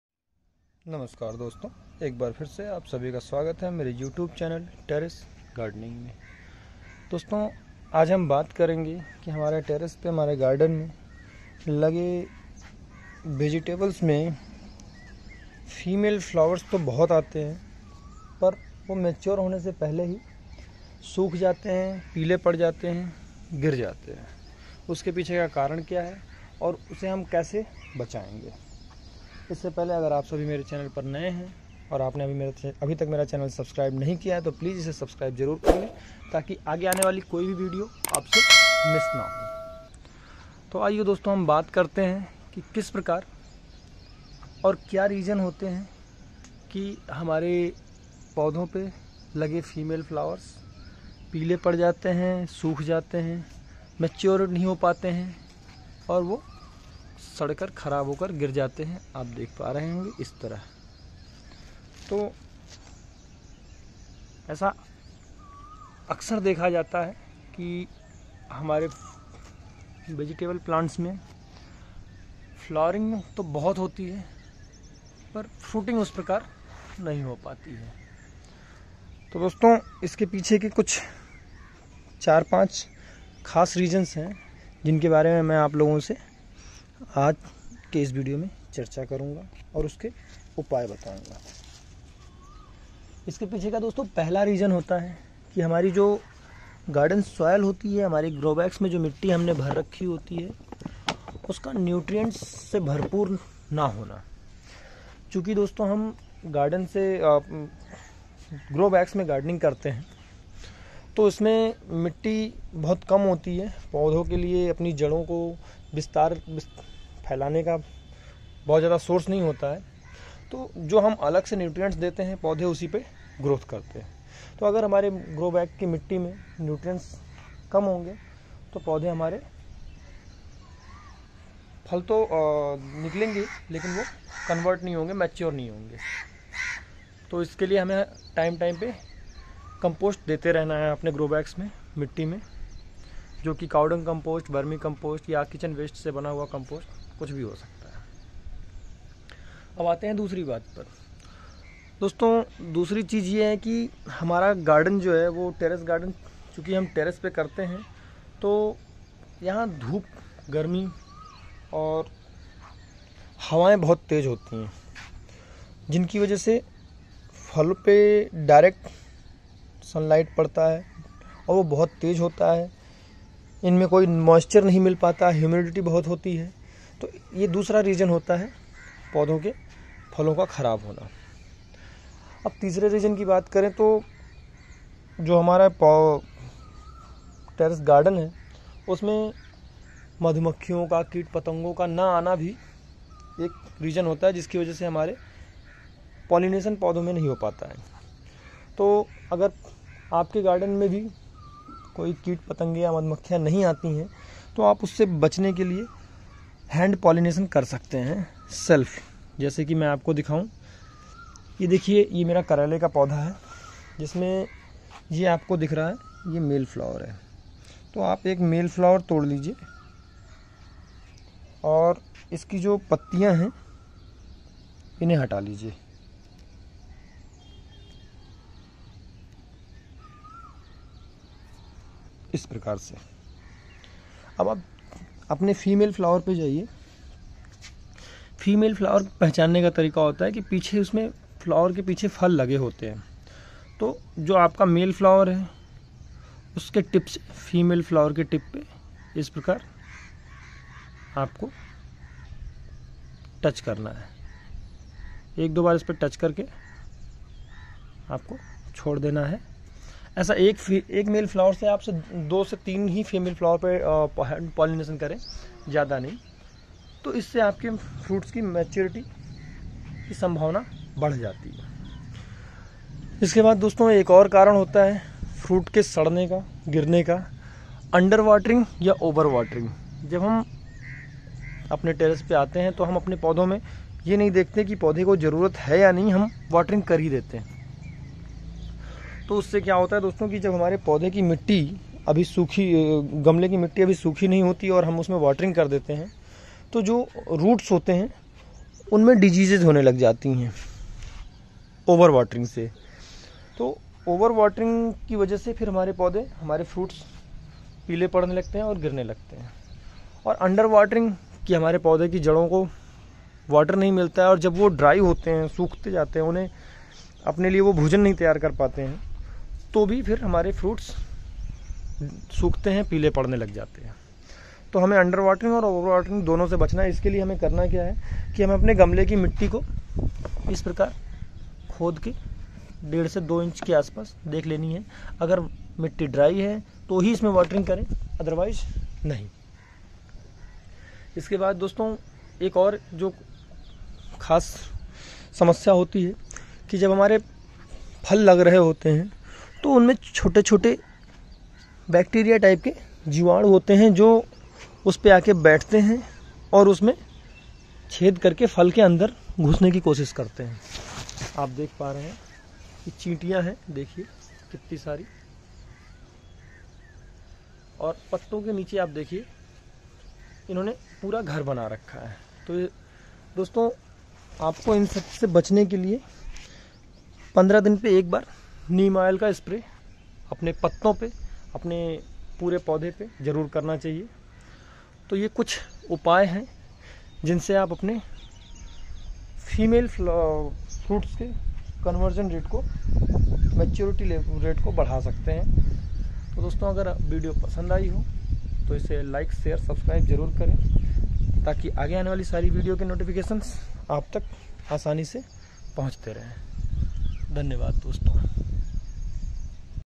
Hamare paudho ke phul kyo jhad jate hai//कीजिए ये organic उपाय फूल कभी नहीं झडेंग।। Why flowers drop
Phul jhad jate hai phal nhi ban pate to ye video aapke liye hi hai.